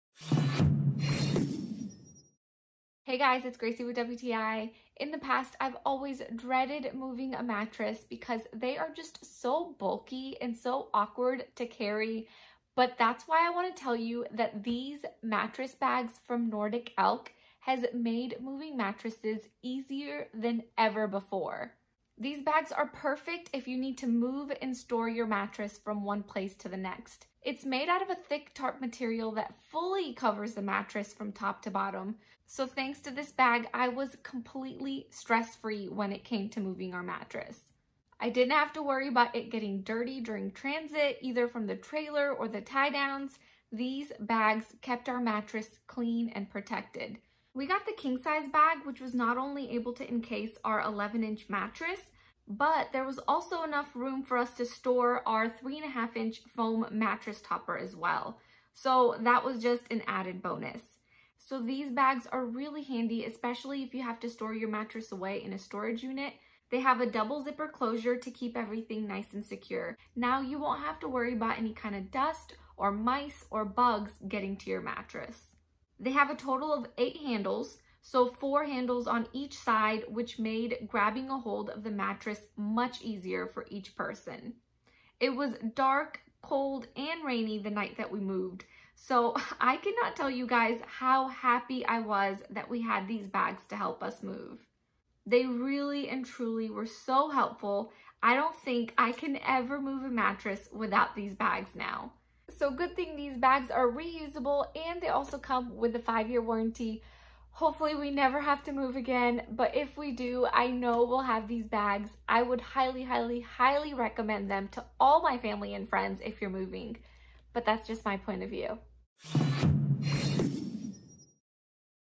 MattSafe Mattress Bags for Moving | Our Point Of View
Check Our New Website For Amazing Deals!
(Commissionable Links)

Polyethylene Tarpaulin
Imported
For moving or storage – Our quality mattress cover will keep your valuable mattress safe.
High-quality thick tarp material – The mattress bag is made of thick tarp material, which will give extra protection to your mattress.
Carry-friendly handles and zipper - Let’s face it, moving a mattress can be a daunting experience. That’s why our mattress bag for moving has an extra-large zipper and 8 strong handles that can easily be carried safely. Put the mattress in the bag, close the zipper and use the sturdy handles to lift it.
Reusable – The mattress bag for storing and moving can be used over and over again. This means that you will both save money and do a good deed for the environment.
5 years warranty - We are so confident you’ll love using our mattress bag that we give 5 years warranty on the purchase. If for any reason you would not be satisfied with the bag, we will issue a full refund.